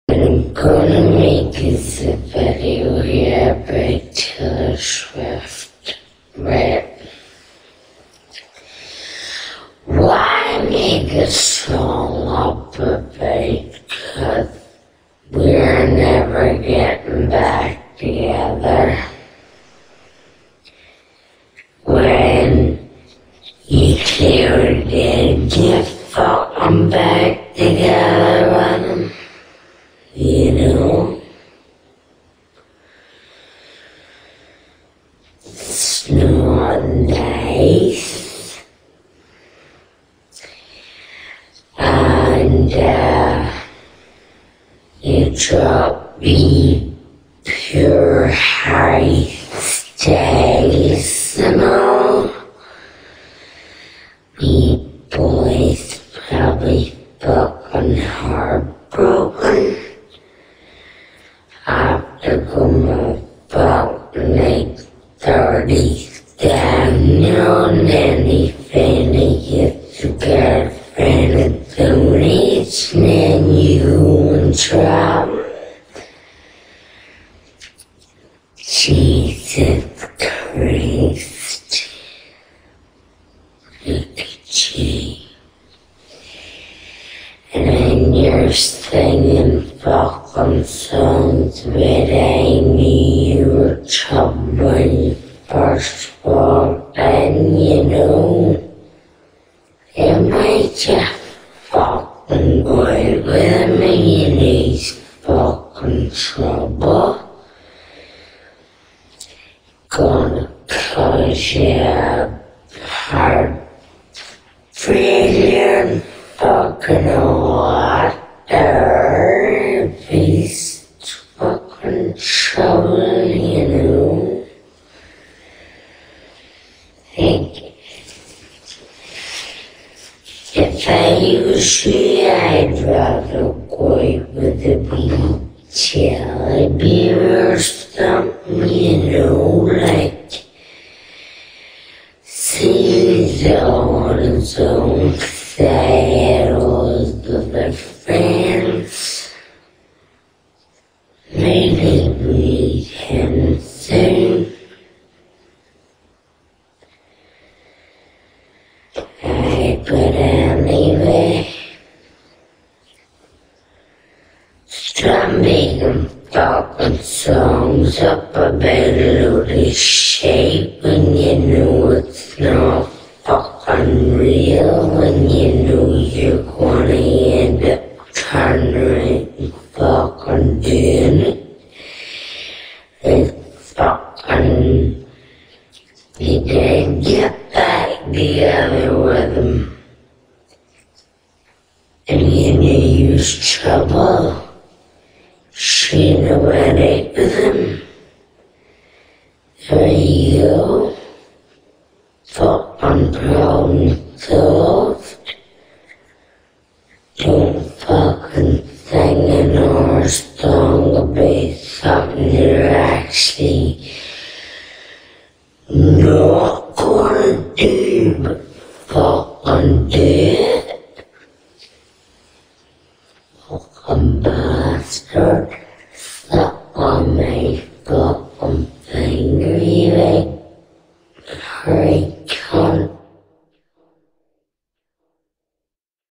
Young girl rants to Taylor Swift (Demon Version)
A young Northern Irish girl gives a rant to Taylor Swift, in a demonic style.